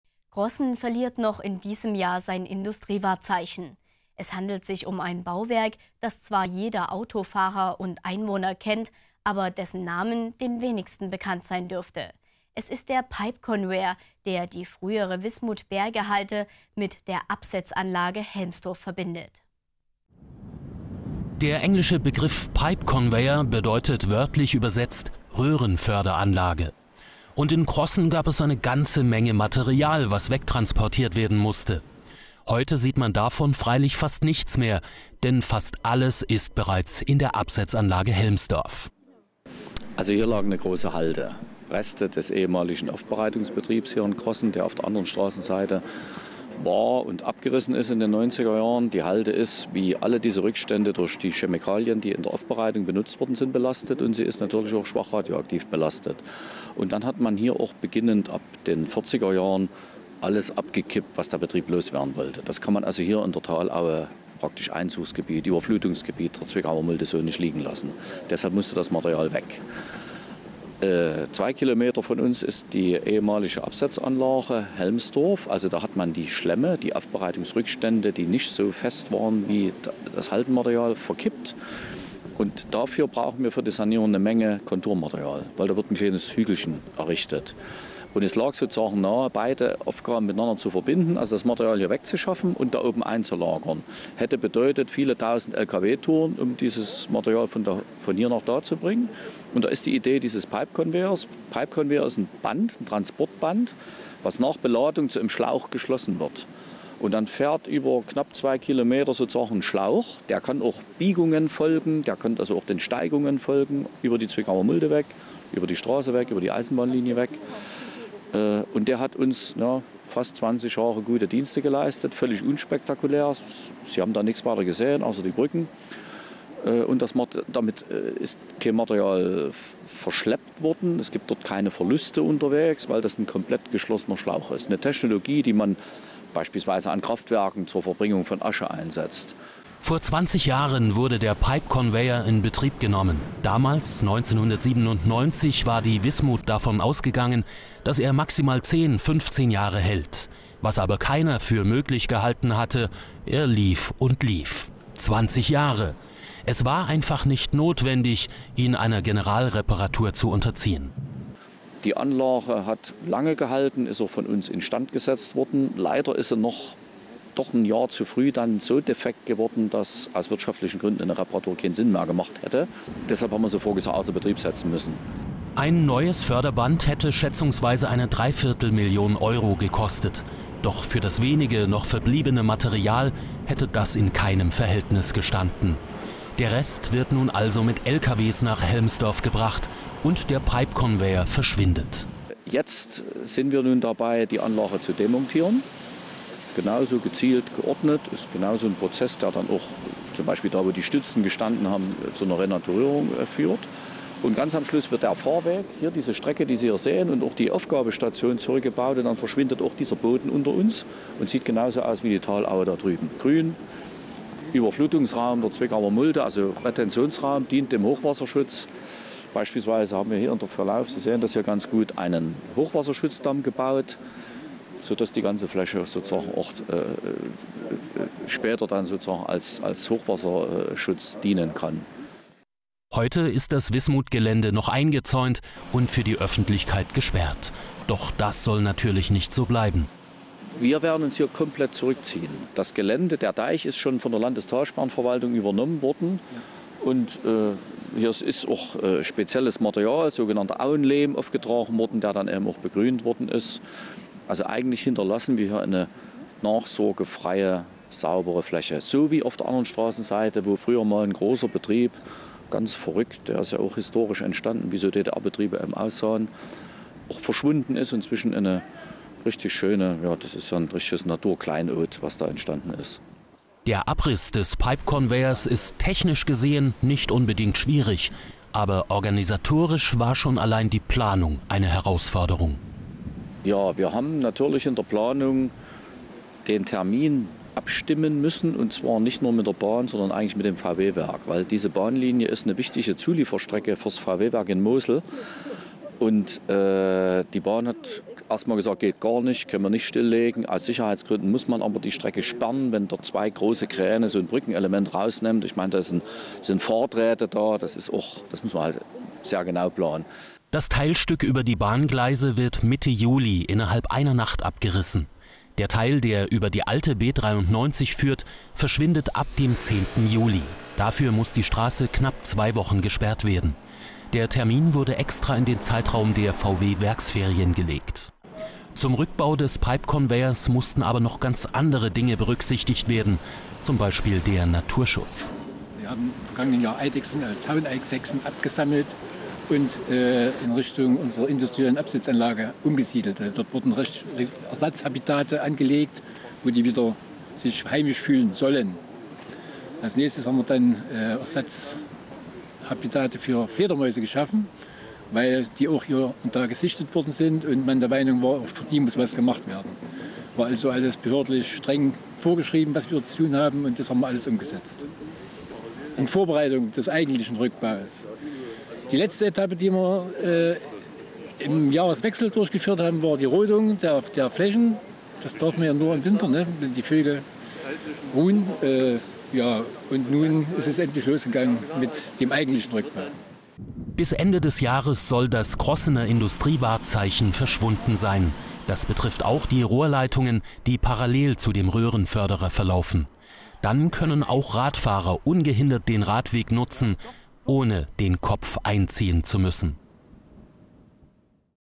Crossen verliert Industriewahrzeichen
Crossen verliert noch in diesem Jahr sein Industriewahrzeichen. Es handelt sich um ein Bauwerk, das zwar jeder Autofahrer und Einwohner kennt, aber dessen Namen den wenigsten bekannt sein dürfte. Es ist der Pipe Conveyor, der die frühere Wismut-Bergehalde mit der Absetzanlage Helmsdorf verbindet.